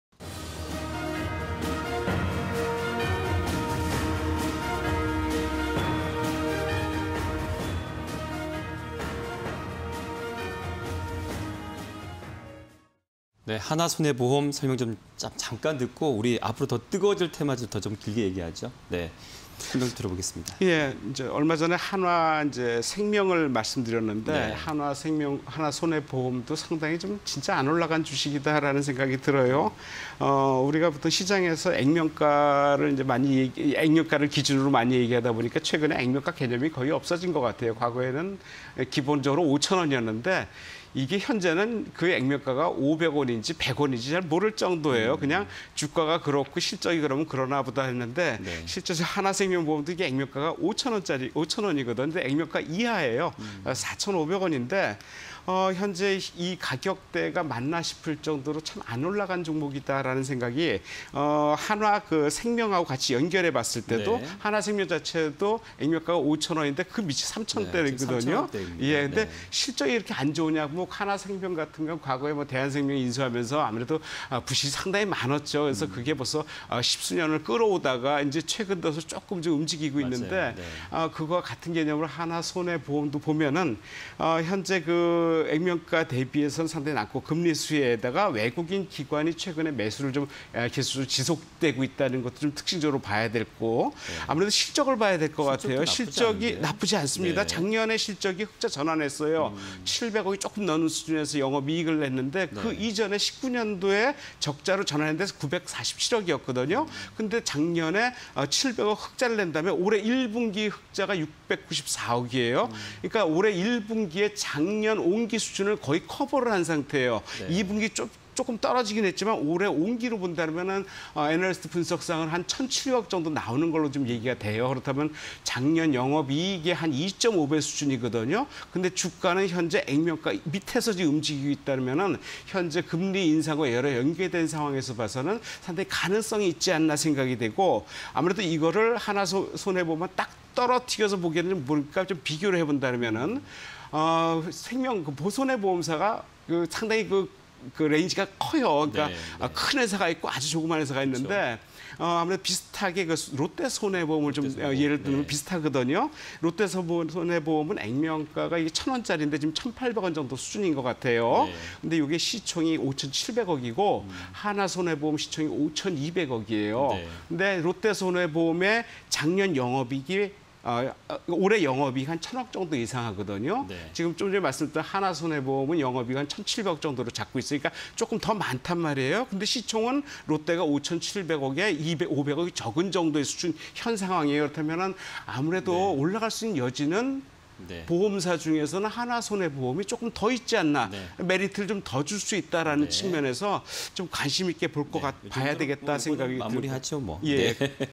한화손해보험(000370)... 금리 인상 관련 수혜 기대_테마IN이슈 (20210929)
한화손해보험(000370)
- 금리 인상 관련 수혜 기대
- 외국인ㆍ기관 동반 매수 유입
- 사상 최대 이익 기대감